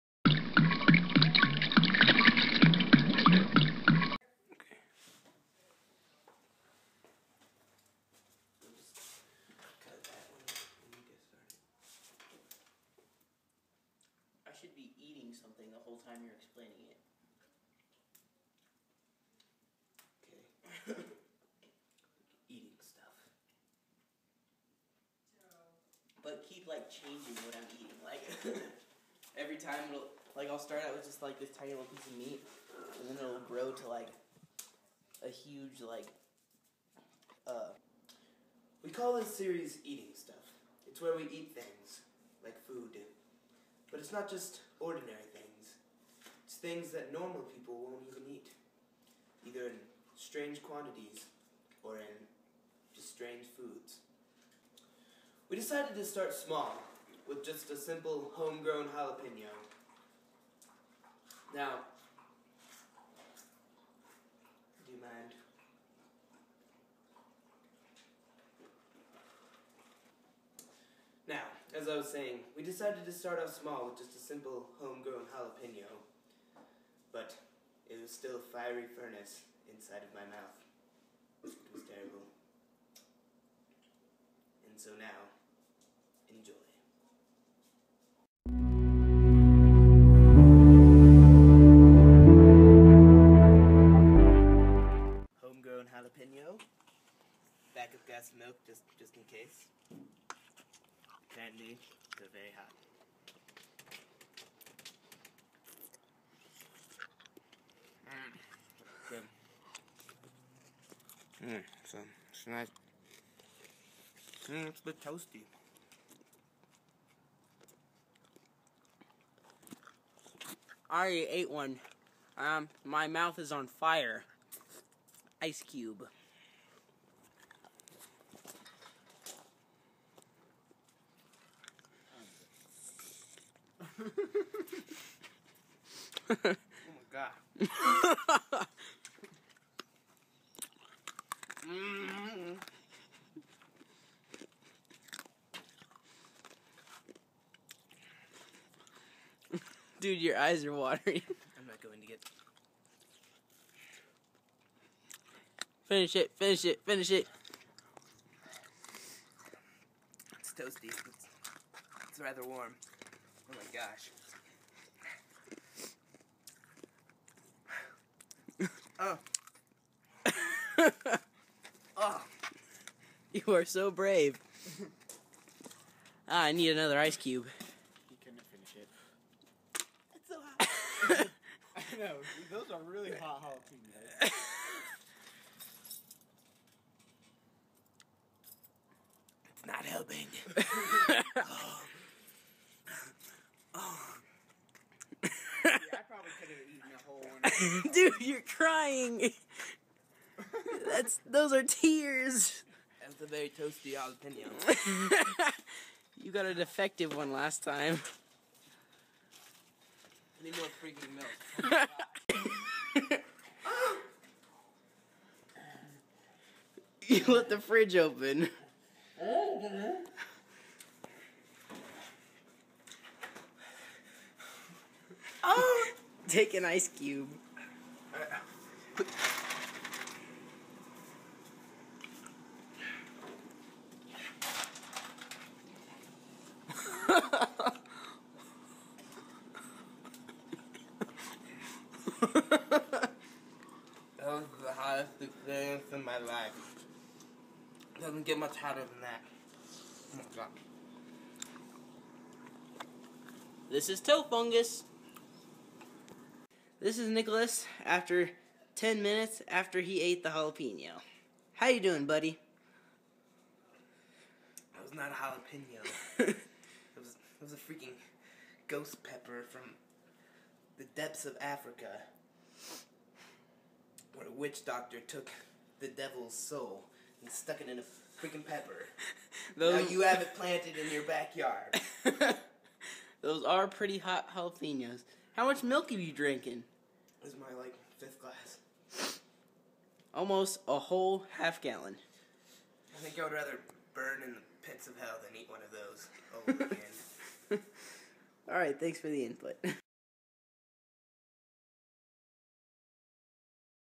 EATING STUFF: Jalepeno
This is the first of many videos. We call them "EATING STUFF". It's where we eat stuff.